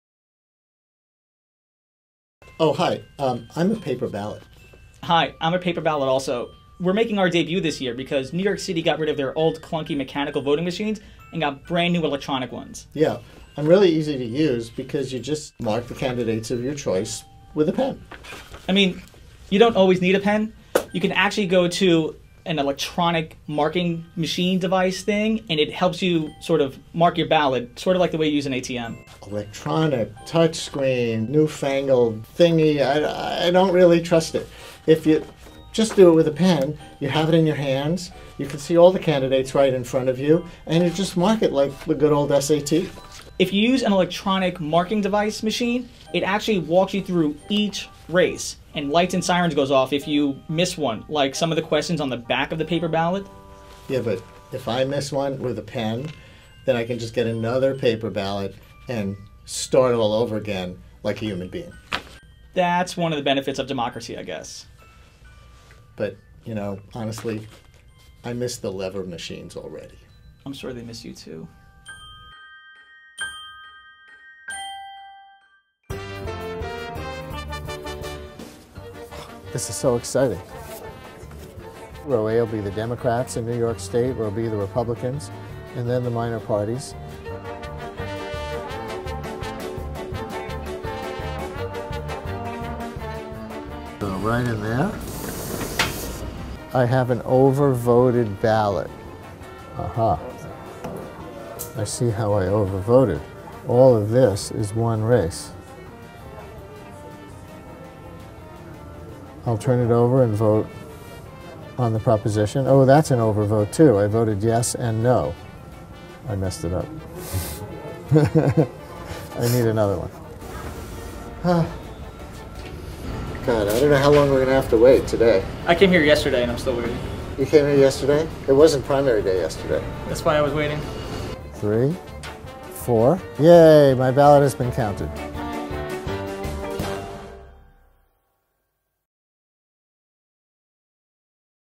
Hi, I'm a Paper Ballot
When voters go to the primary polls on September 14, they will use New York's new paper ballots for the first time. WNYC's Brian Lehrer and Azi Paybarah recently took the new ballot for a test drive.